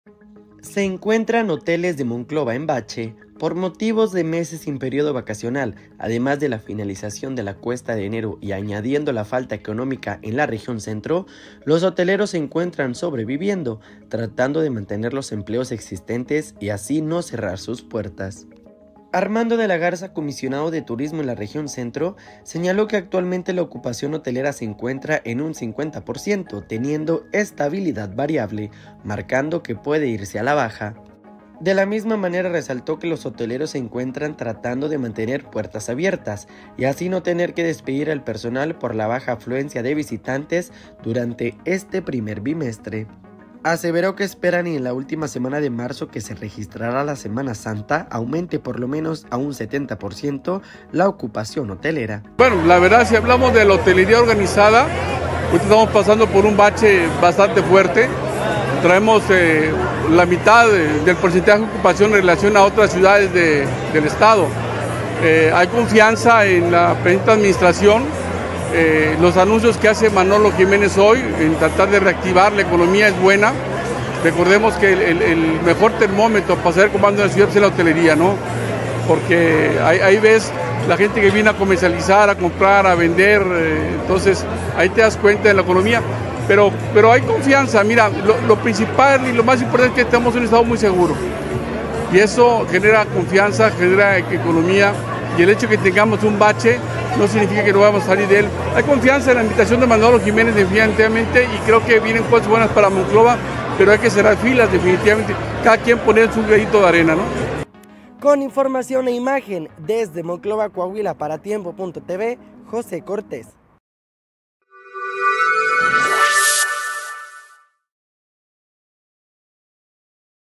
Hoteleros caen en un ‘bache’ por economía de Monclova
¿Cómo afecta la falta de vacaciones, la cuesta de enero y la situación económica a los hoteles de Monclova? 🏨 En este video te mostramos cómo los hoteleros se enfrentan a este reto y qué medidas están tomando para sobrevivir y conservar los empleos. No te pierdas esta nota de interés.